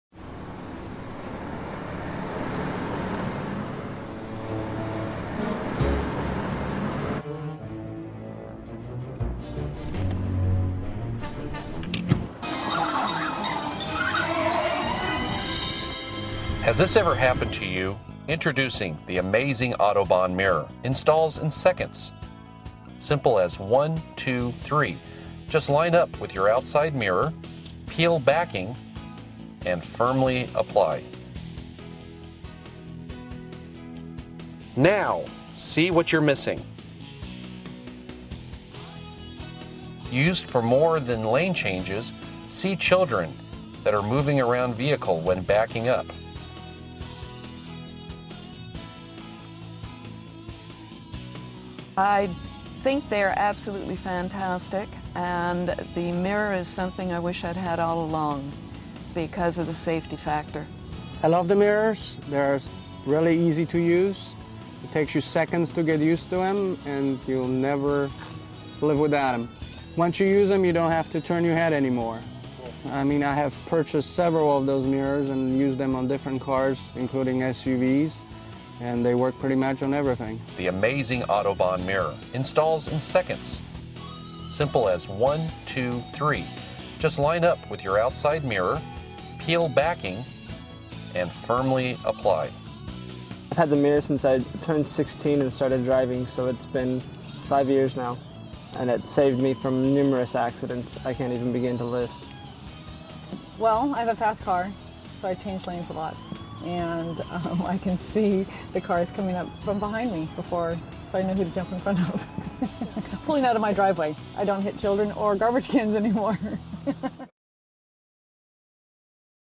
The Amazing Autobahn Mirror BLIND SPOT MIRROR
AMAZING AUTOBAHN BLIND SPOT MIRROR IS Made in Germany, TUV approved. This unique 3 oz. mirror will eliminate dangerous blind spots! What makes this mirror stand out is that it attaches INSIDE the vehicles windshield, increasing drivers vision by 5X.  With the ability to see whats in the blind spots without turning ones head and keeping the drivers eyes in the forward direction of the vehicles travel. This New mirror conceivably could save hundreds of dollars of claims and even lives!

FEATURES:
The Amazing Autobahn Mirror lets you quickly and clearly oversee the traffic situation without turning your head. Thus seeing what cars are in front of driver instantly.
Rearward/Side vision increased by 5X fold
Variable length support arm adapts to all types of vehicles, including Trucks, Motorcycles, Heavy Equipment, and Aviation.
Two socket-joints allow individual adjustment for every driver.
Spherically bent surface increases the visual range 5 fold. Ratio is 1:1. No magnification.
The smooth and gradual curvature of the mirror produces no irritating breaks or double images and is virtually distortion free.
The Amazing Autobahn Mirror is non-blinding at night.
Mounts inside the vehicle making it not susceptible to outside elements blocking view such as rain, fog and snow.
It is Shatter free and Reusable by simply replacing the adhesive backing. Making the Amazing Autobahn Mirror usable for many years of use.

TUV (Technischer Uberwachungs-Verein) An organization approved for testing products to VDE standards. US-based companies often use TUV in place of VDE because they have established facilities in the US.

VDE (Verband Deutsher Electrotech niker) A German organization that sets standards for product safety and nose emissions; VDE tests and certifies products to those standards.

Click  Home  Detailing Supply Store Driver Safety Products